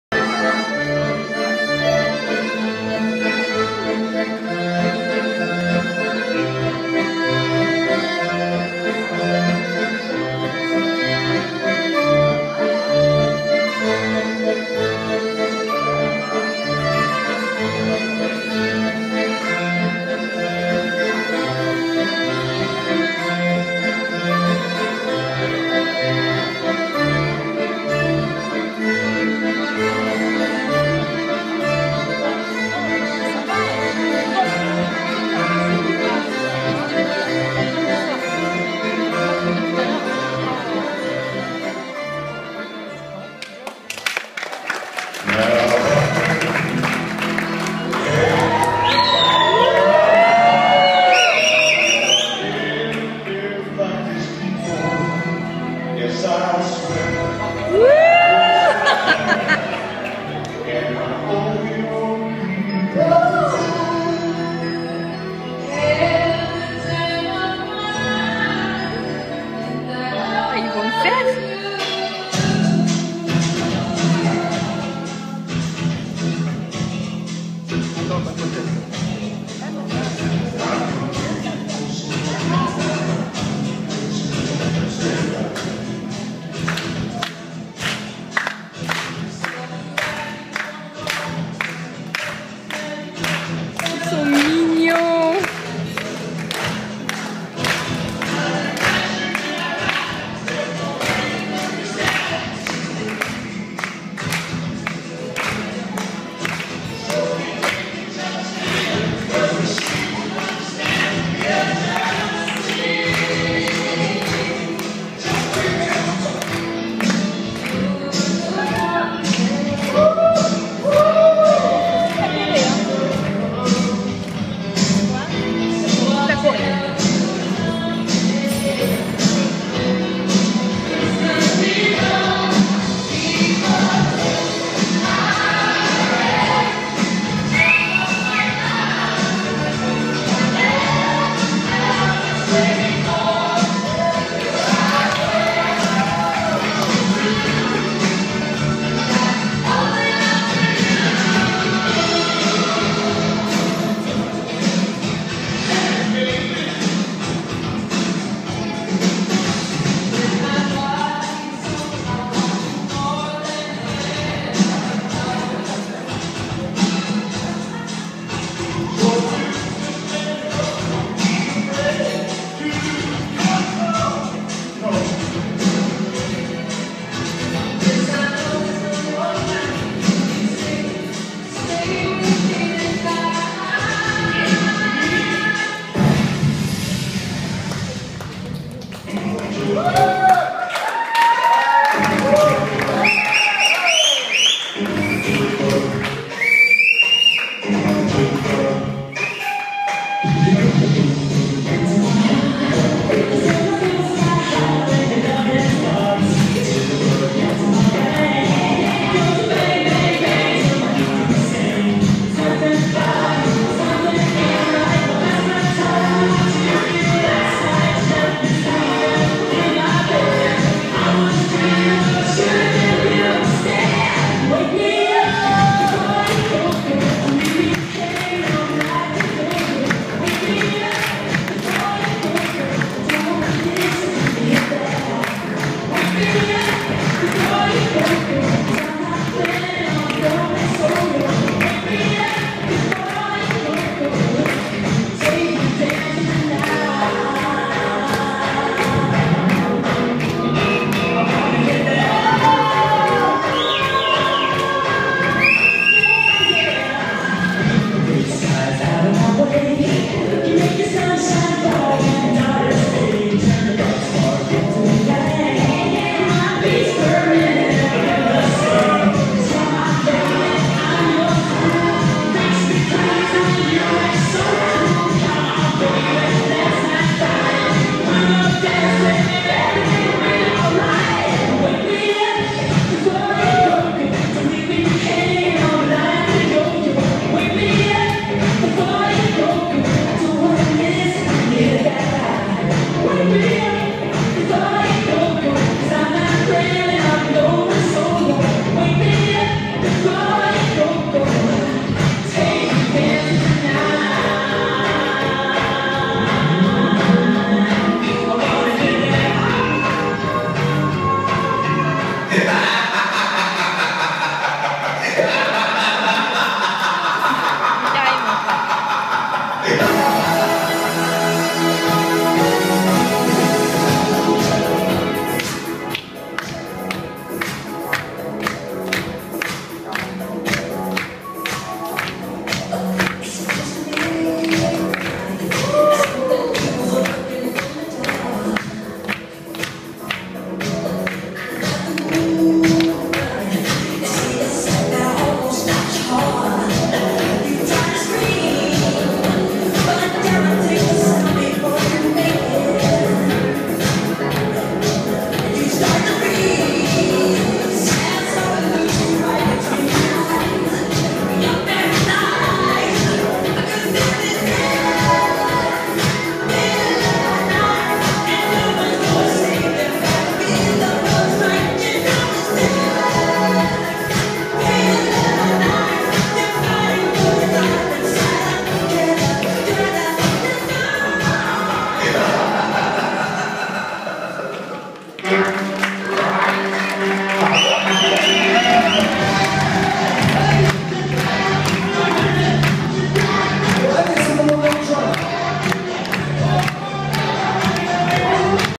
Ouverture bal Carole et Franck (2013)
Ouverture de bal du mariage de Carole et Franck
Eté 2013

Préparer aussi votre mariage avec Levidanse...